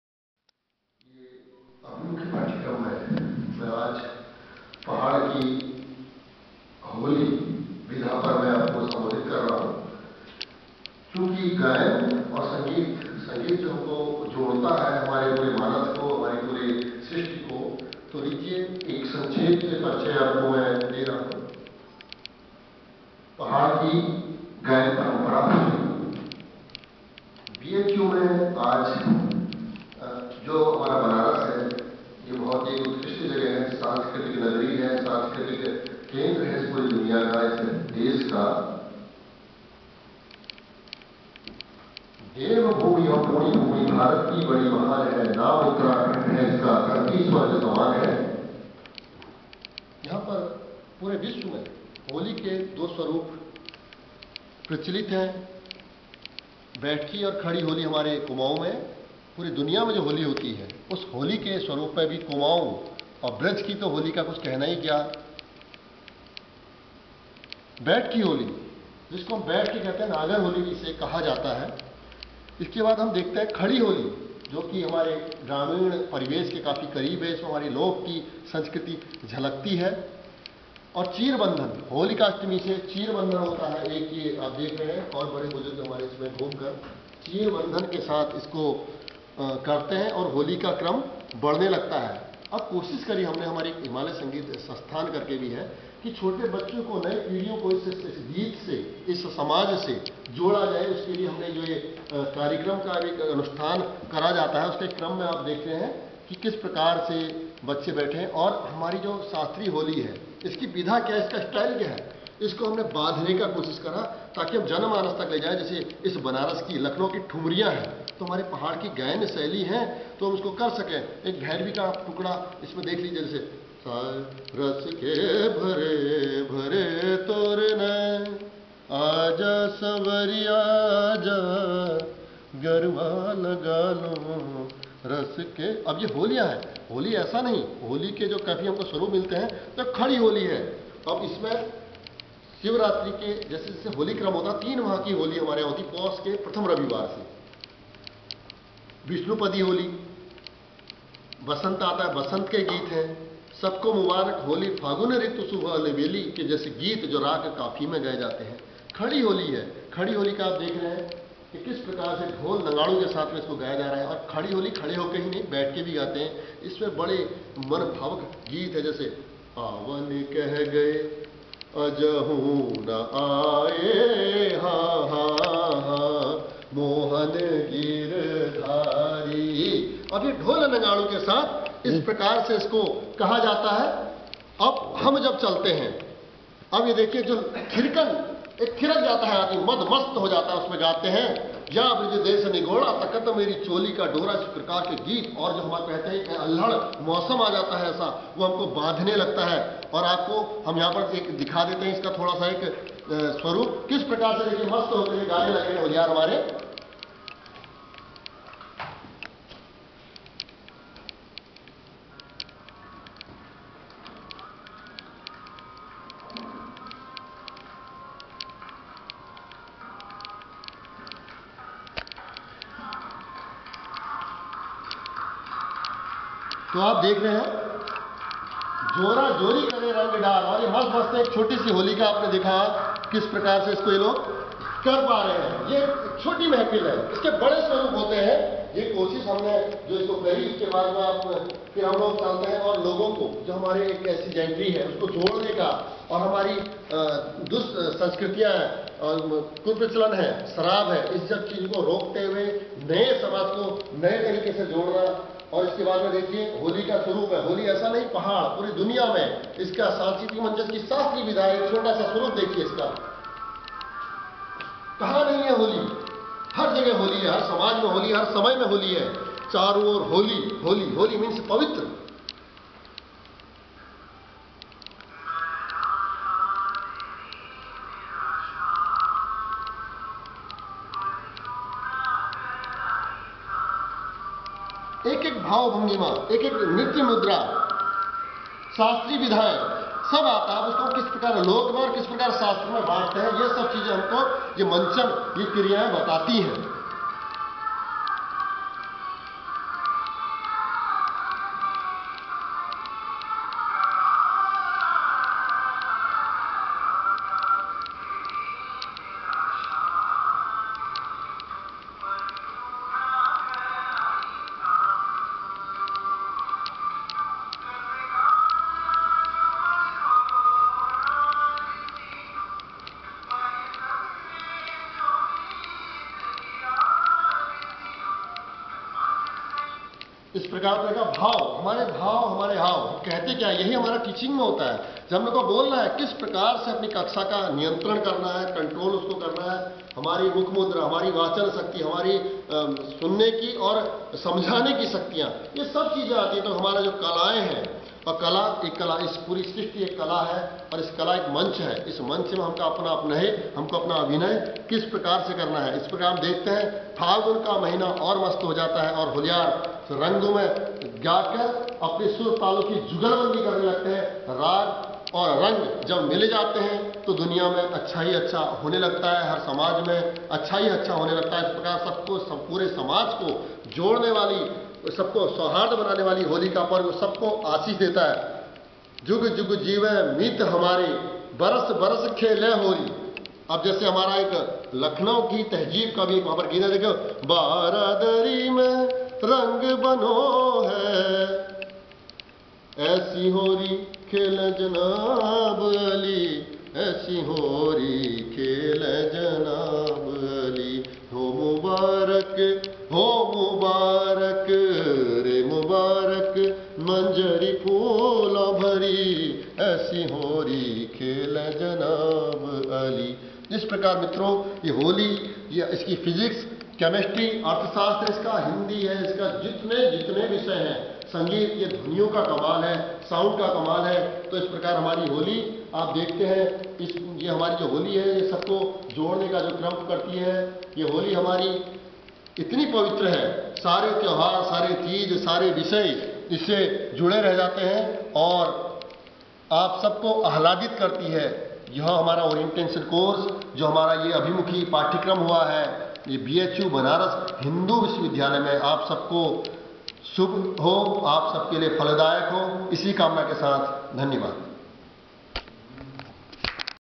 Pahad ki Holi by Pankaj Upreti
On 6 January 2015-2 February 2015 Dr.Pankaj Upreti has a lecture on "Orientation Coures"and the organiser is UGC -Academic Staff College BHU.